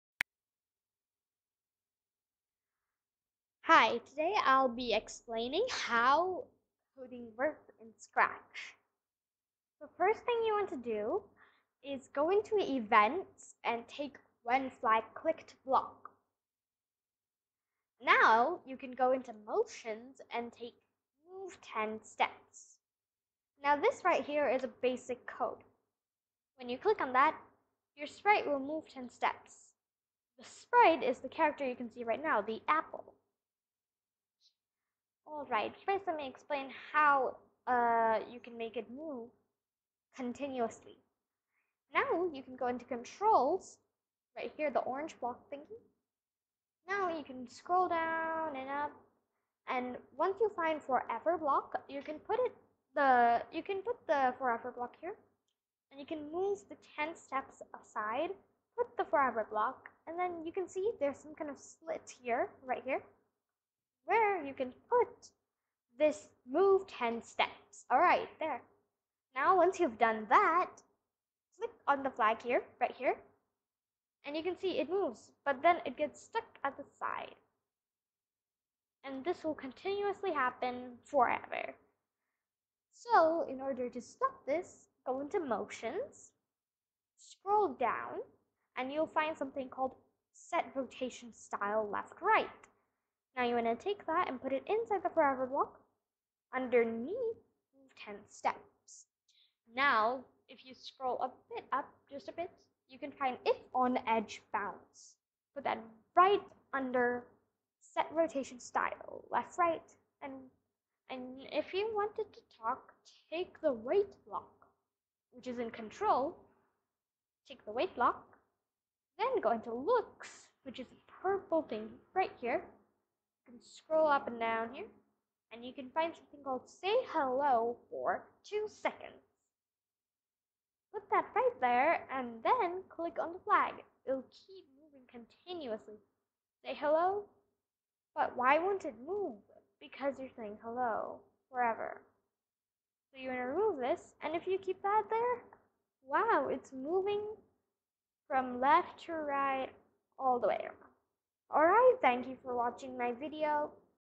HOW TO MAKE A MOVING APPLE 🍎 GAME I FREE CODING for KIDS I CLASS#1 I
"Uncover the secrets of a fulfilling life through captivating coding adventures and inspirational insights with the Life Coding Magician!"
FREE CODING for KIDS I CLASS#1 I HOW TO MAKE A MOVING APPLE 🍎 GAME.

Everyone’s talking about coding for kids and coding for begginers, but where do you start? This video series will give you an introduction to the world of code and basic concepts. By the end, you’ll know whether Data Science, Computer Science or Web Development is right for you as a coding begginner.

best regards
Coding Magician